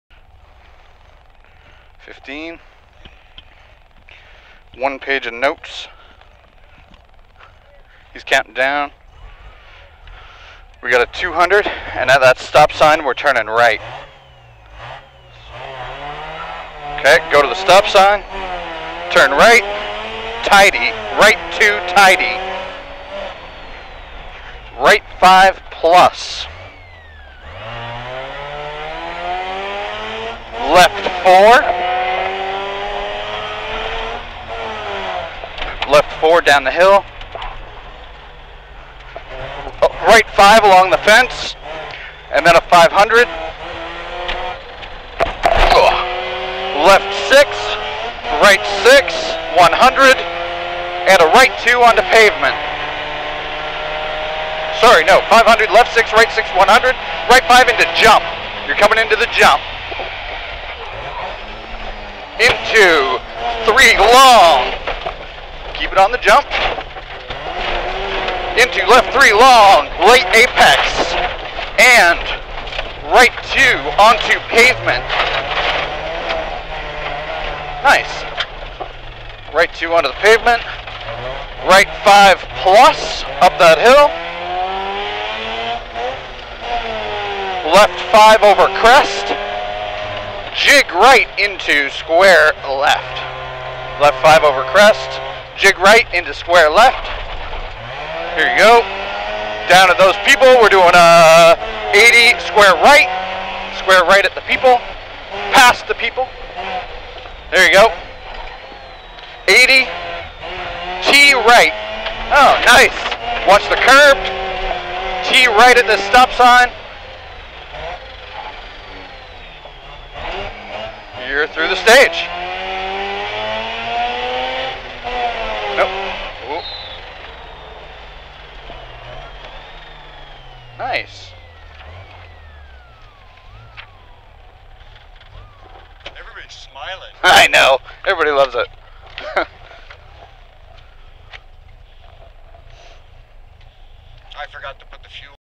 Rally Charlevoix 2015 - La Malbaie 1 2015
Car #23, Swann / Dine Rallye Charlevoix

First stage of the event.  Dine's first regional event, first regional event together.